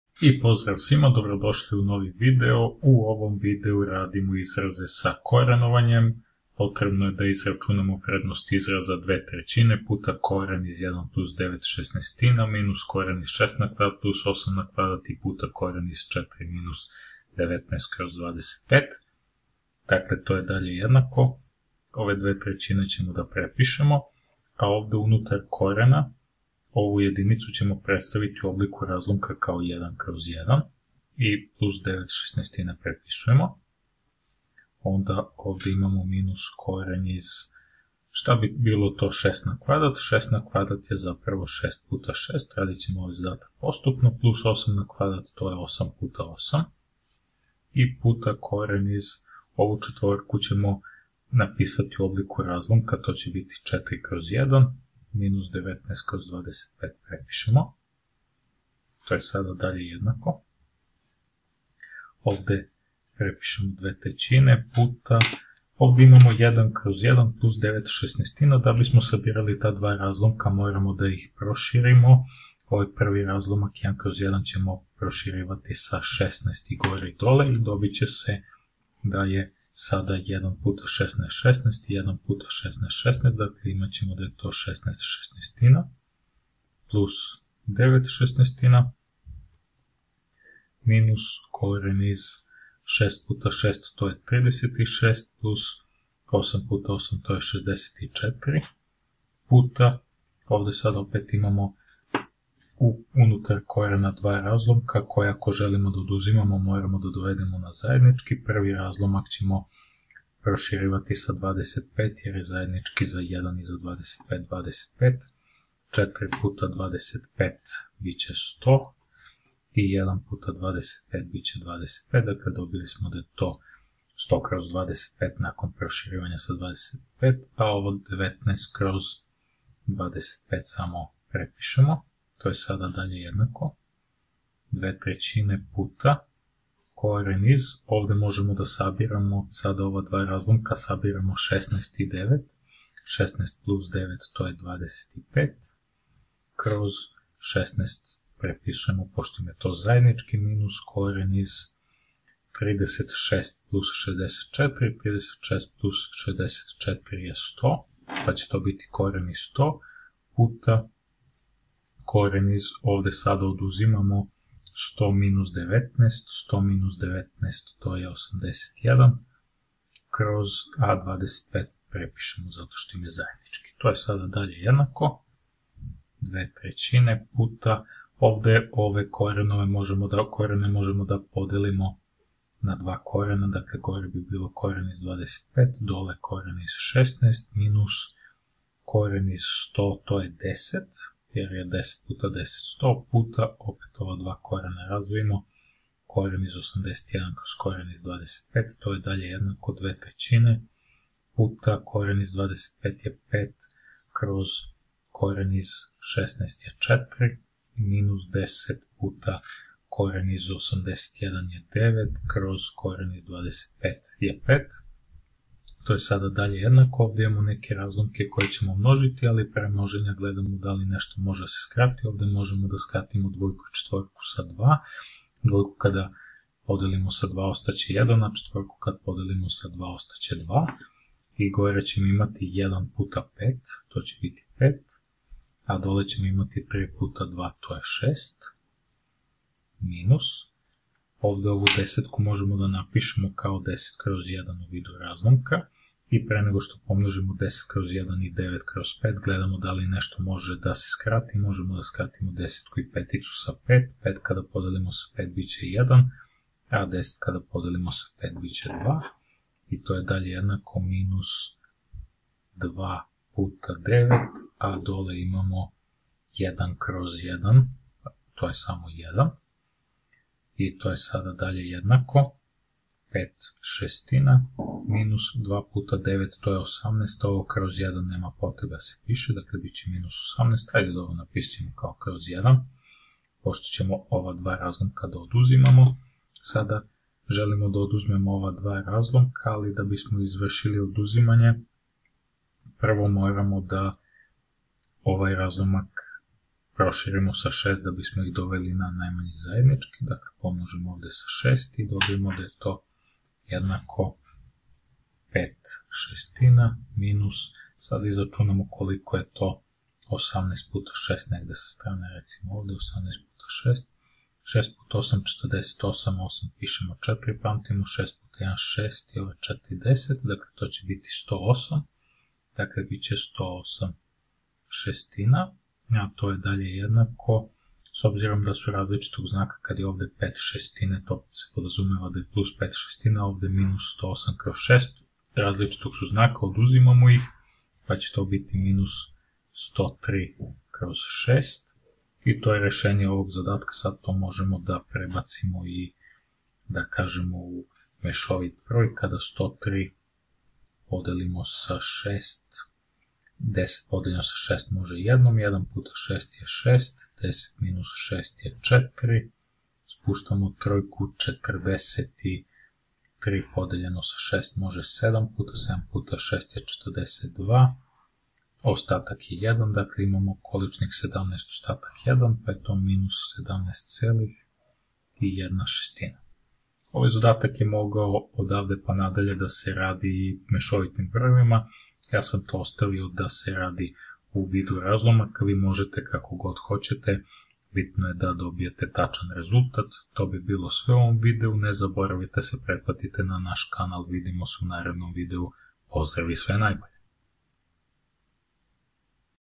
Korenovanje
U ovom videu rešavamo zadatak izraz sa korenovanjem.

Matematika za 7 razred.
Matematika za 2 razred srednje škole.